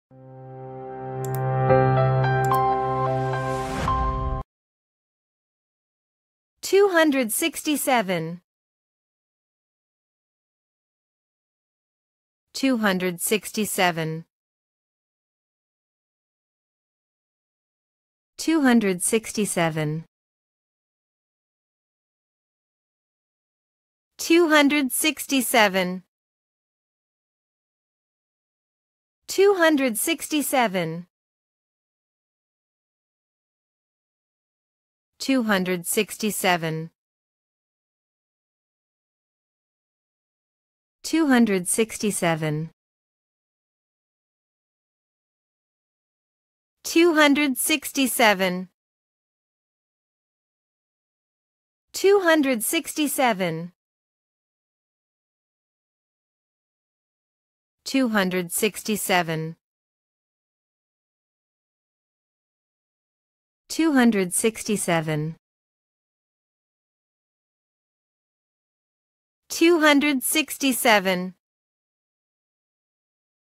How to Pronounce 267 in American English Correctly
How to Say (267). Listen to the (Two hundred sixty-seven) audio pronunciation in English.

Cardinal numbers are numbers that are used for counting. They are also known as natural numbers or cardinals. A set of cardinal numbers starts from 1 and it goes on up to infinity. We use cardinal numbers to answer the question "how many?".

For example, how many students are going to the school picnic?